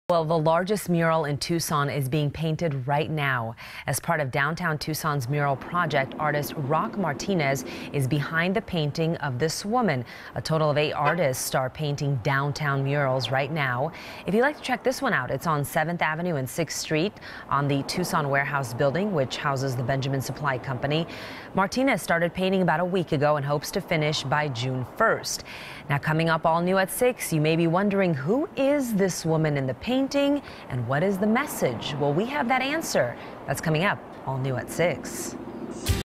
Largest mural in Tucson being painted
The largest mural in Tucson is currently being painted.